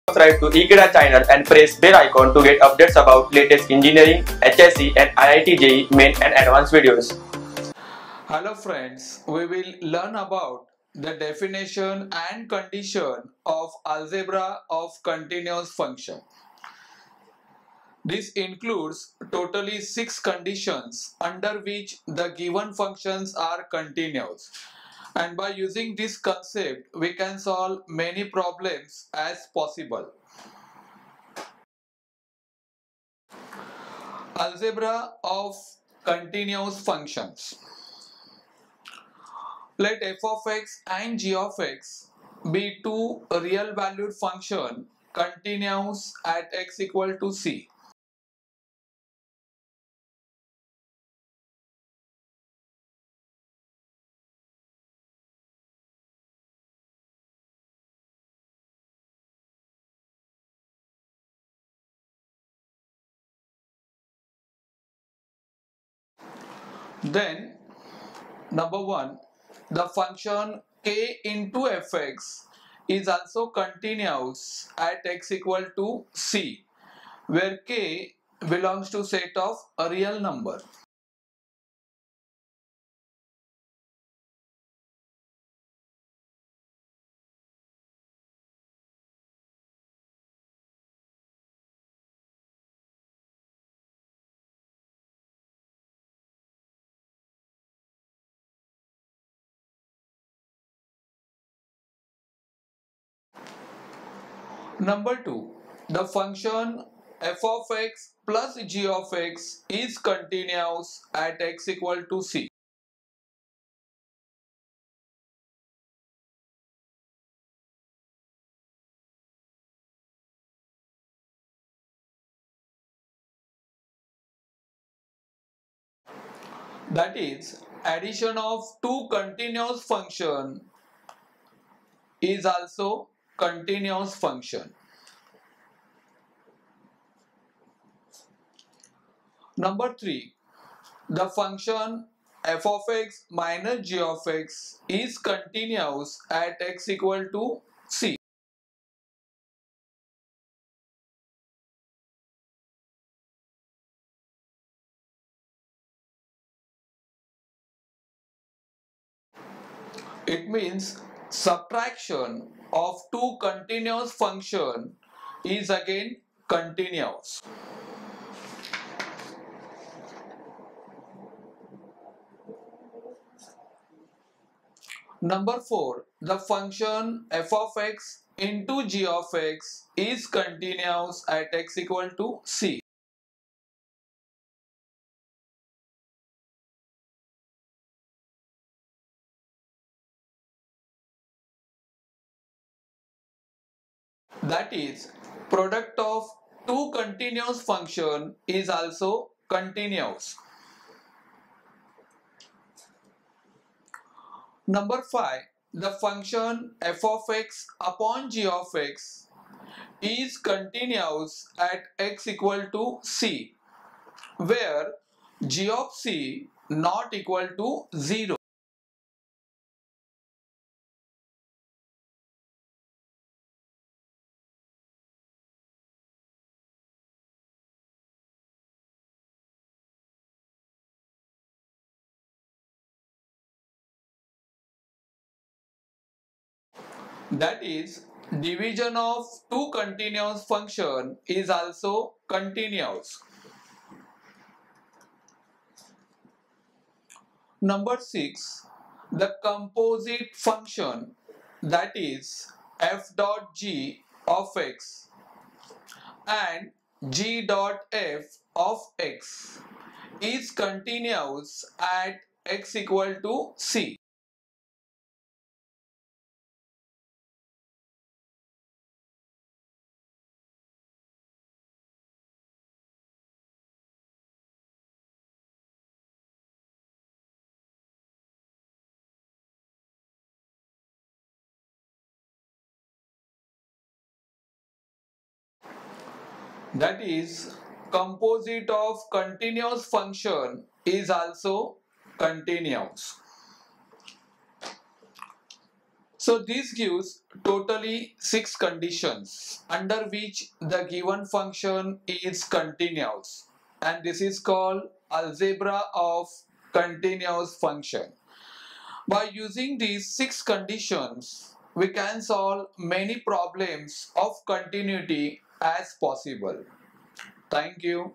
Algebra of Continuous Functions - Continuity - Mathematics Class 12
Algebra of Continuous Functions Video Lecture from Chapter Continuity in Mathematics Class 12 for HSC, IIT JEE, CBSE & NEET.

Happy Learning : )